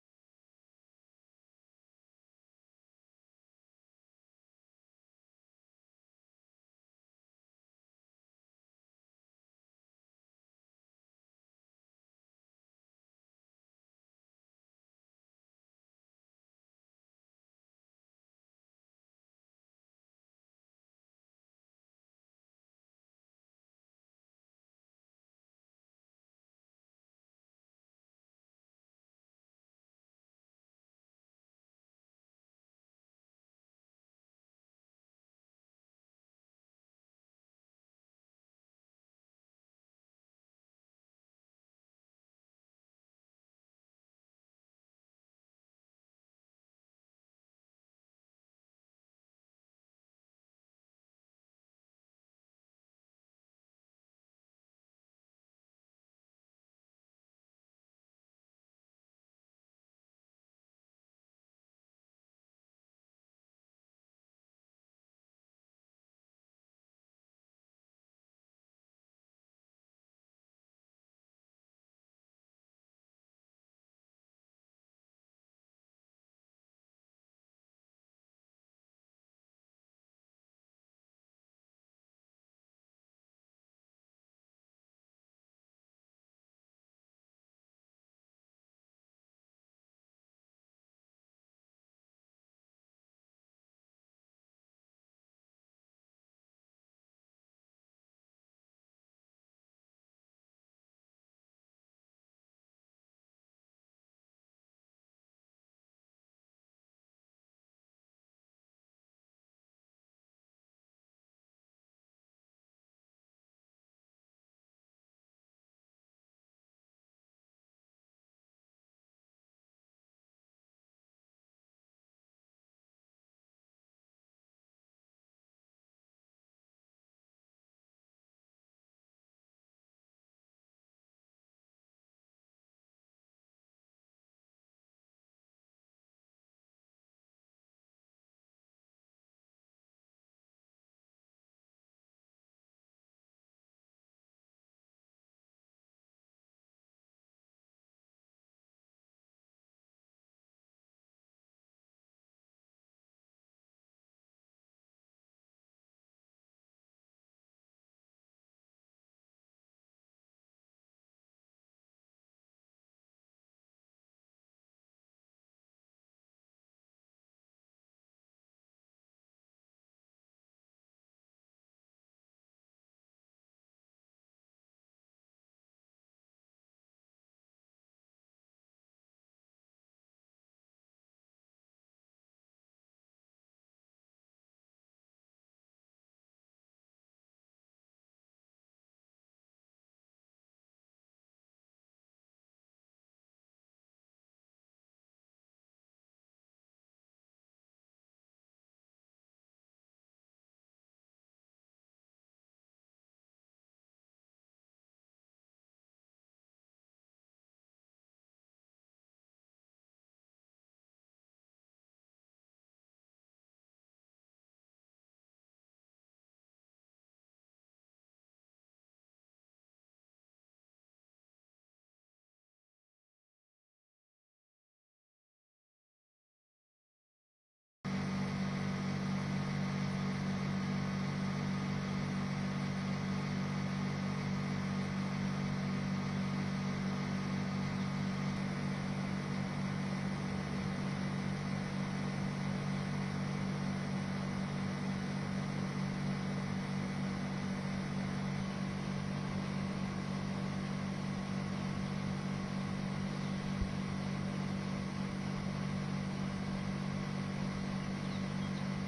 Drone shows destruction in Jenin as Israeli raid continues | ABS-CBN News
Roads and buildings were seen heavily damaged on Monday (September 2) in the city of Jenin during a widespread Israeli military operation in the occupied West Bank.

Hundreds of Israeli troops have been carrying out raids across the West Bank since last Wednesday (August 28) in one of their largest actions in the area in months, which Israel says is aimed at rooting out Iranian-backed Islamist militants.

The operation, now in its sixth day, has caused heavy damage to infrastructure in the city and the adjacent refugee camp, a densely populated township, with multiple damaged buildings and a mass of streets torn up by armored bulldozers hunting for roadside bombs.

Palestinian health authorities said at least 20 Palestinians had been killed during the operation.